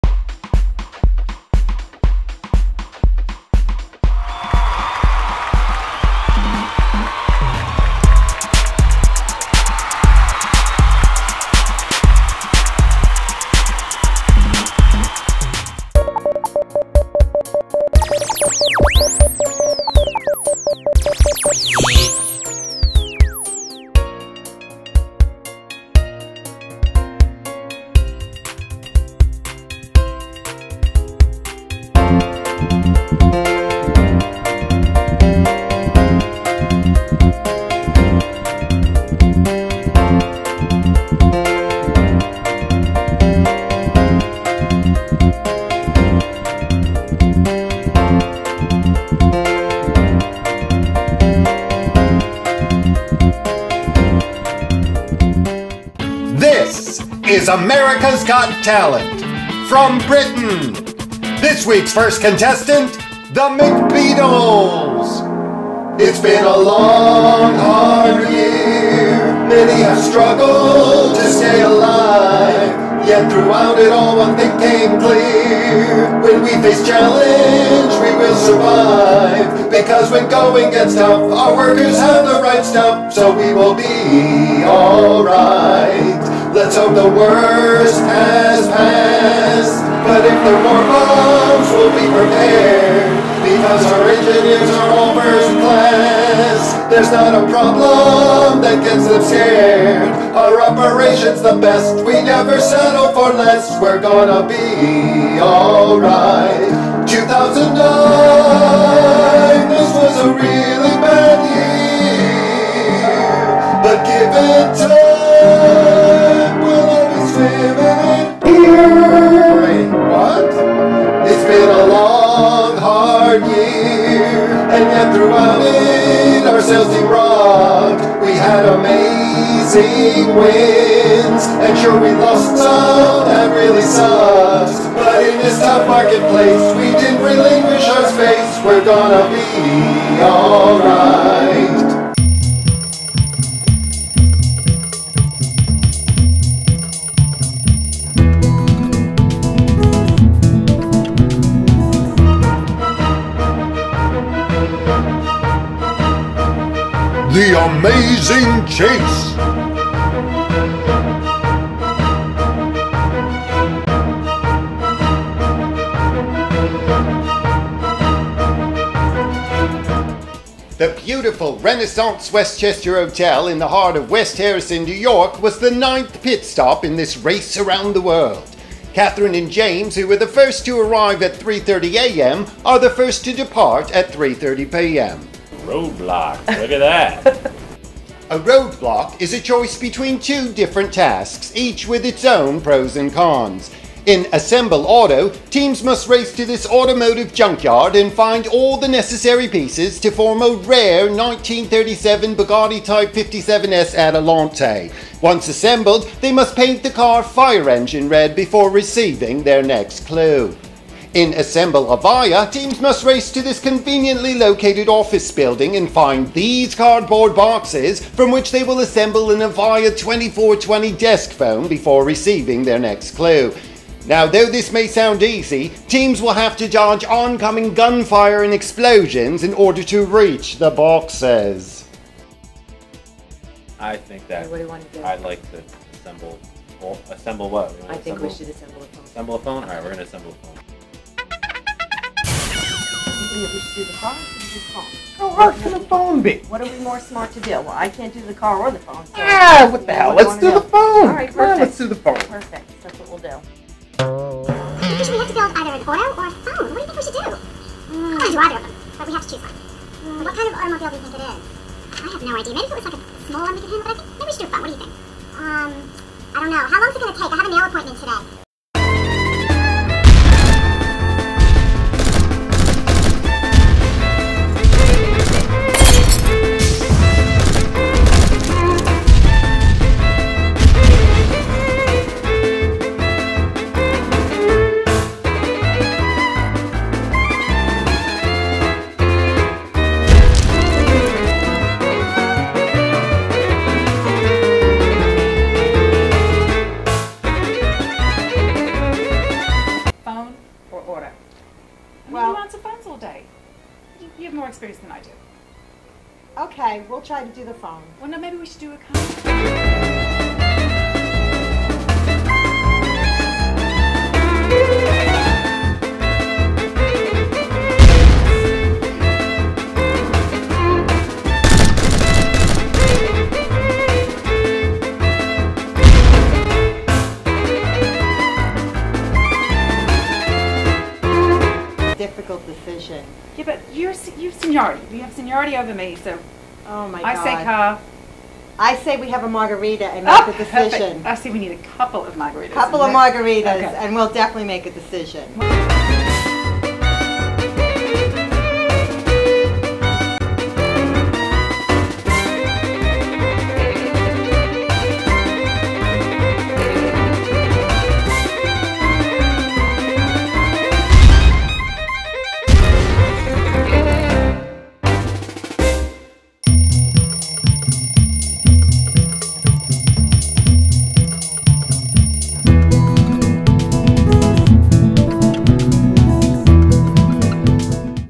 Rock-n-Reality, PART ONE -- CTI Holiday Video 2009
PART ONE. On December 2, 2009, Consolidated Technologies held it's annual Year End Party and Awards Presentation. The theme of the party was "Rock-n-Reality" and this video was created to humorously poke fun at the last year, as well as to look forward to the future with cautious optimism. The video also parodies many popular reality shows, including "America's Got Talent," "The Amazing Race" and "The Apprentice."  The video has been split into two parts in order to fit on YouTube.